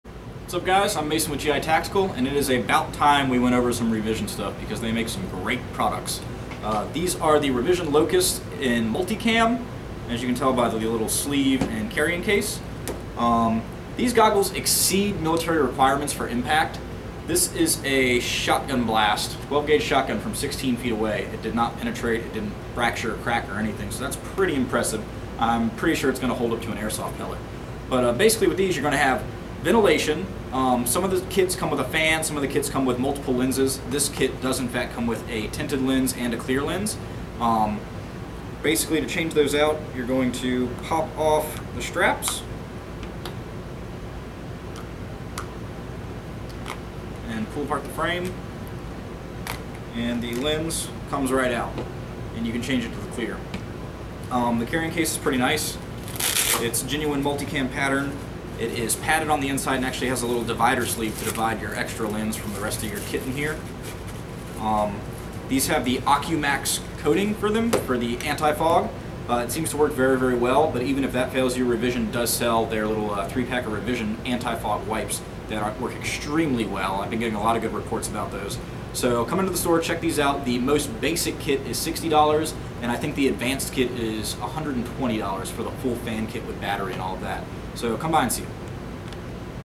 Airsoft GI Uncut - Revision Desert Locust Goggles
Maison from GITactical reviews the Revision Desert Locust Goggles and explains how to replace the lenses.  Check them and other Revision products out on our website, AirsoftGI.com.